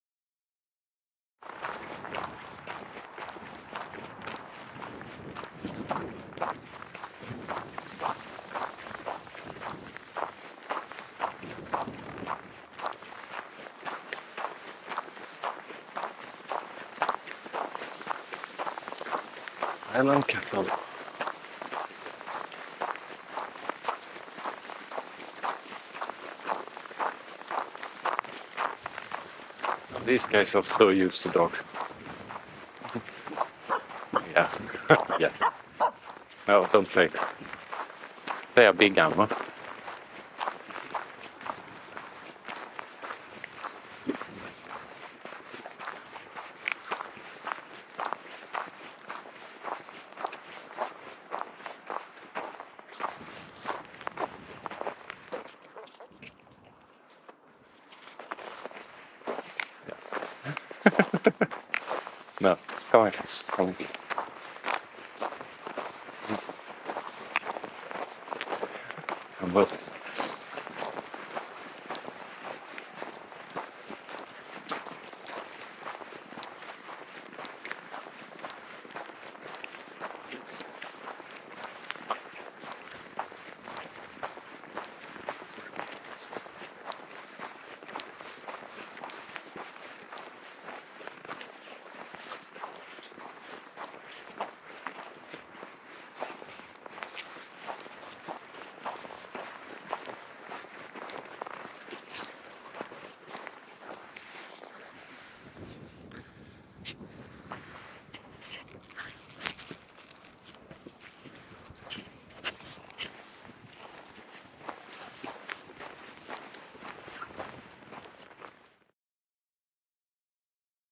Cow Adjacent
Taking a walk down to Välen and Önnered. We met with the Highland Cattle which always interests Aamu.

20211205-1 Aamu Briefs